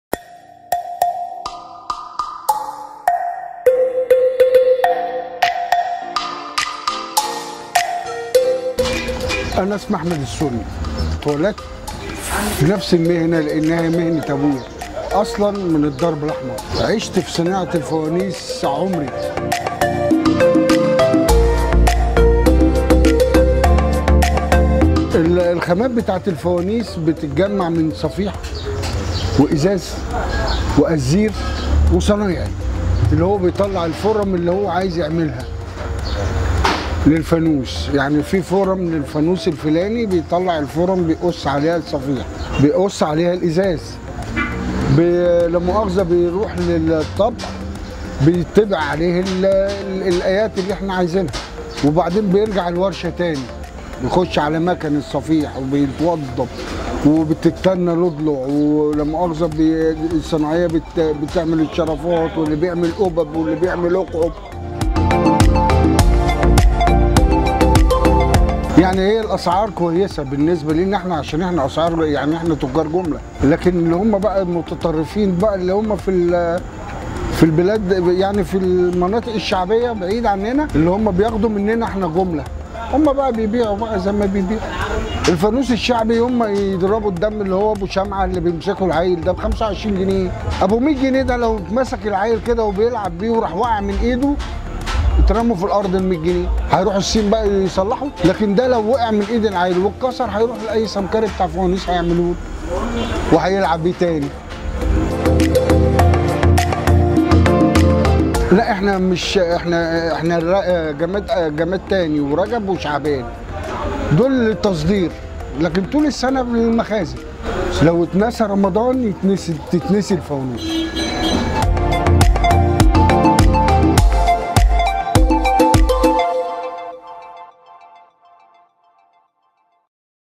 مباشرTV | صناعة الفانوس.. الخامات والأسعار والمستورد
تصنيع الفانوس.. مهنة تورث ويصعب الأستغناء عنها